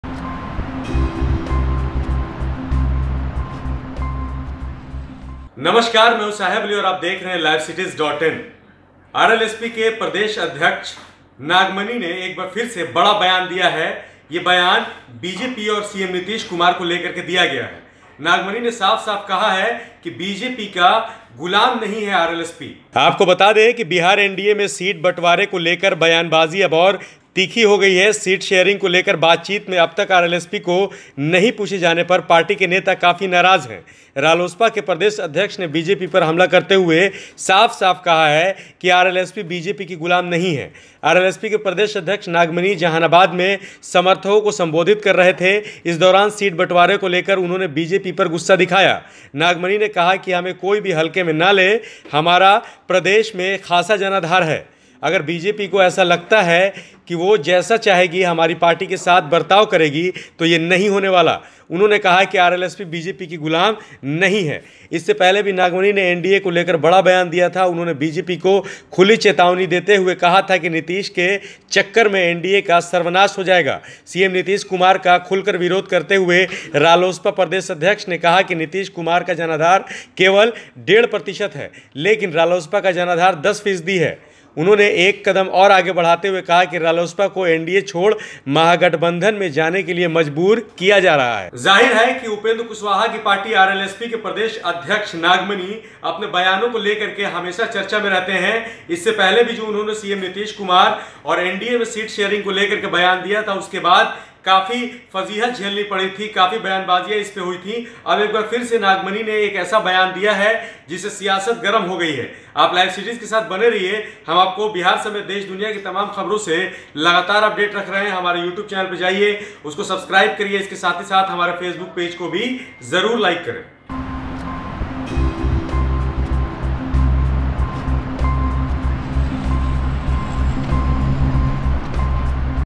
RLSP के Nagmani ने NDA सीट शेयरिंग पर फिर दिखाया रौद्र रूप, बीजेपी की गुलाम नहीं है आरएलएसपी
RLSP के Nagmani ने NDA सीट शेयरिंग पर फिर दिखाया रौद्र रूप, बीजेपी की गुलाम नहीं है आरएलएसपी | LiveCities